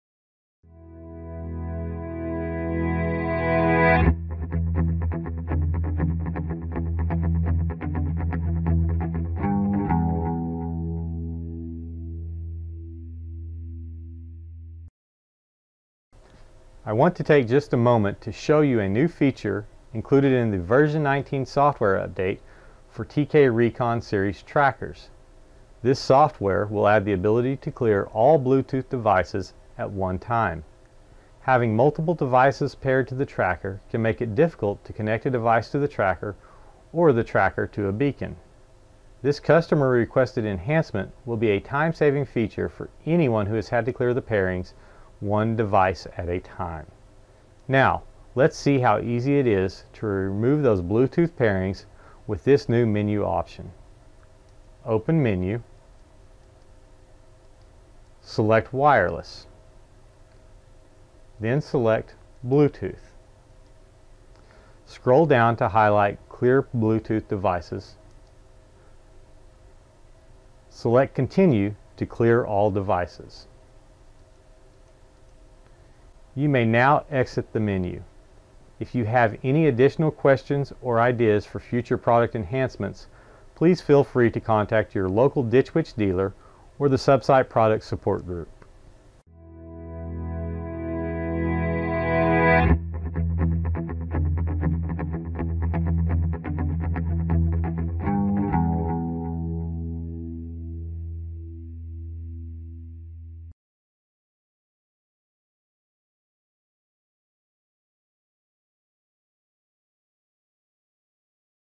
TK Recon Bluetooth Clear
This new menu selection added in software version 19 and later provides the ability to clear all Bluetooth devices at once.